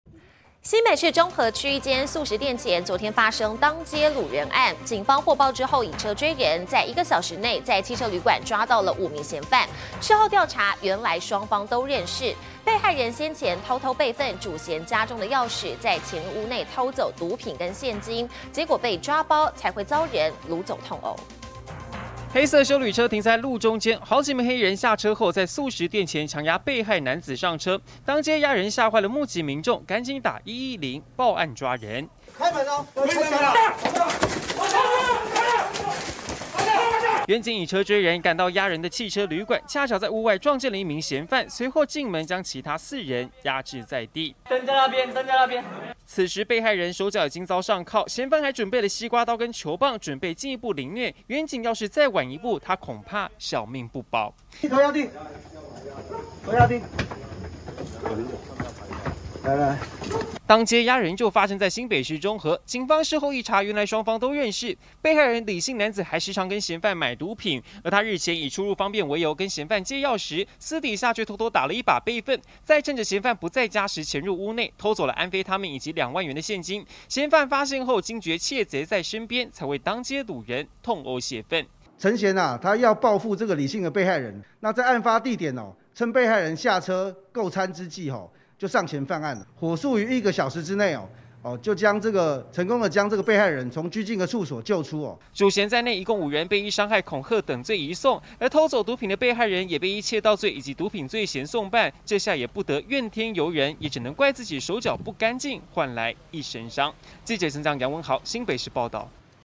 偷毒品又偷2萬現金 被害人慘遭拘禁毆打
毒販好友黑吃黑，遭擄走凌虐。新北市中和區一間速食店前，昨天（17日）發生當街擄人案，警方獲報後以車追人，1個小時內就在汽車旅館抓到5名嫌犯，事後一查原來雙方都認識，被害人先前偷偷備份主嫌家中鑰匙，再潛入屋內偷走毒品跟現金，結果被抓包，才會遭人擄走痛毆。